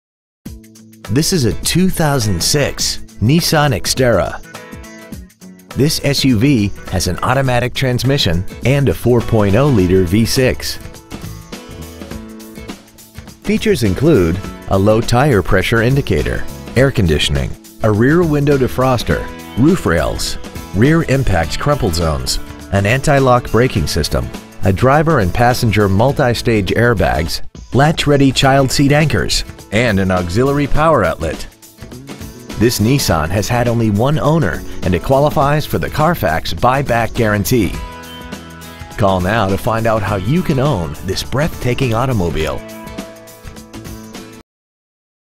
Used 2006 Nissan Xterra Mesa AZ 85207
We have been honored to serve the Mesa AZ area , we promise that your experience at our dealership will exceed your expectations !

 Year : 2006
 Make : Nissan
 Model : Xterra
 Engine : 4.0L DOHC 24-valve V6 engine
 Trans . : Automatic
 Exterior : Black
 Miles : 95,906
 Interior : Gray